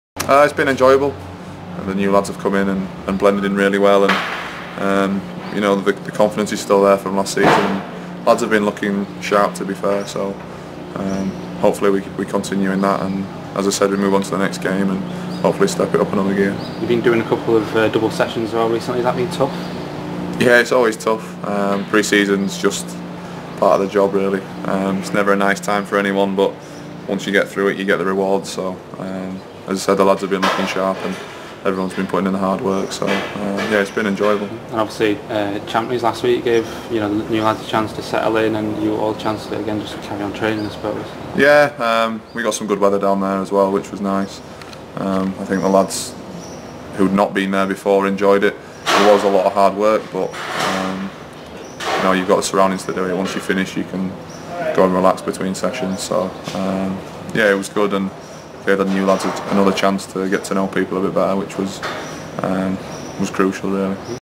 Niall Canavan on the Iron's pre-season so far
Niall Canavan talks about how pre-season is going for his squad.